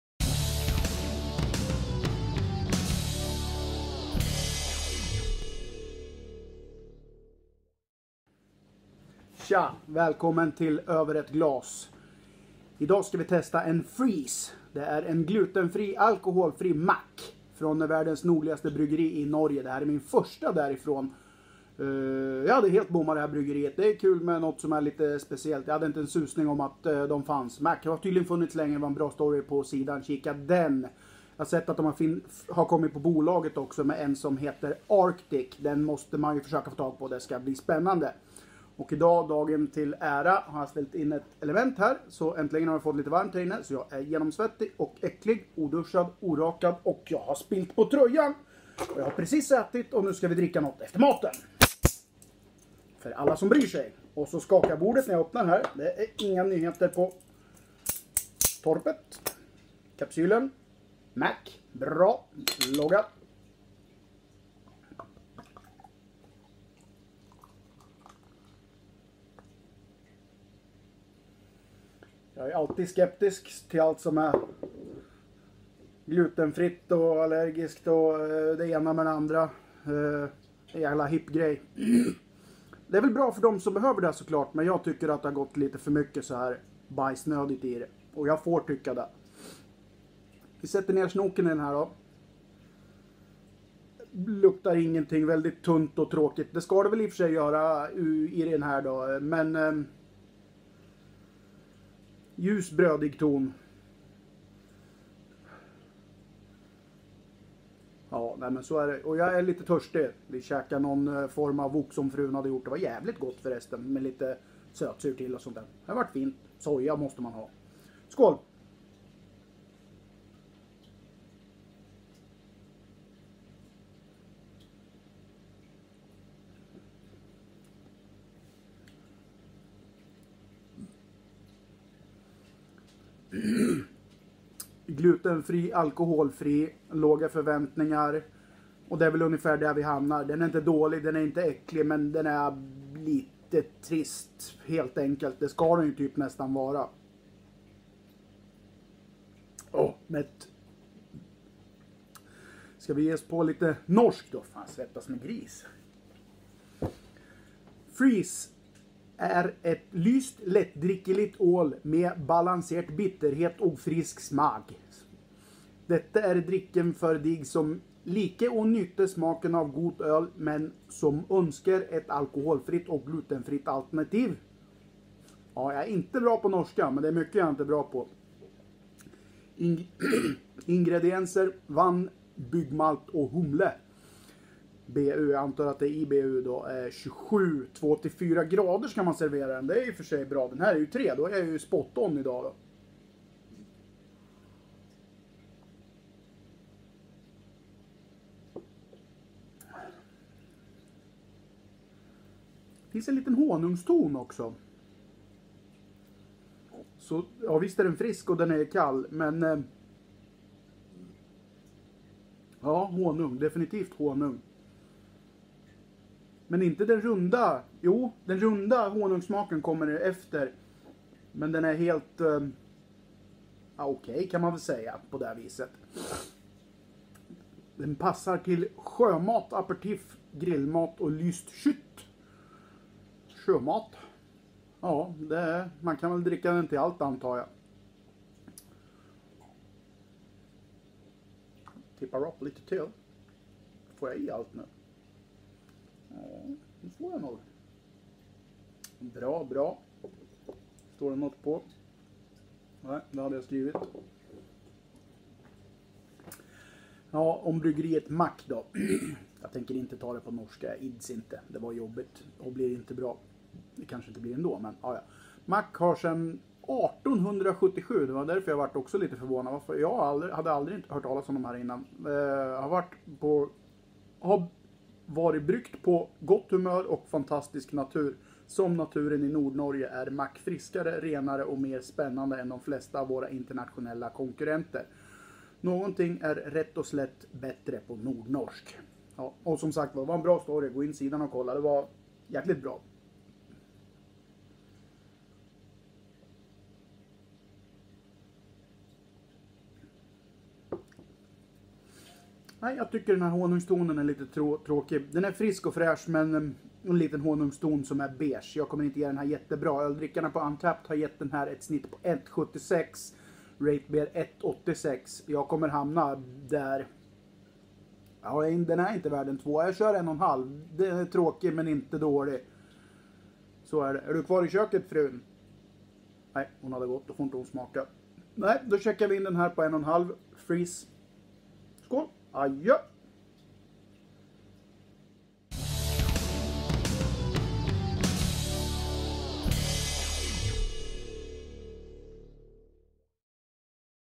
Över Ett Glas #189 Freeze, Mack
Idag testar jag min första från världens nordligaste bryggeri!